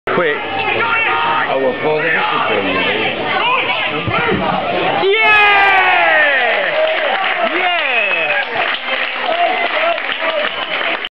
Croydon Athletic 1 Wellingborough Town 1 (FA Cup)
Danny Waldren's superb free kick gives the Rams the equaliser in our 3-1 win.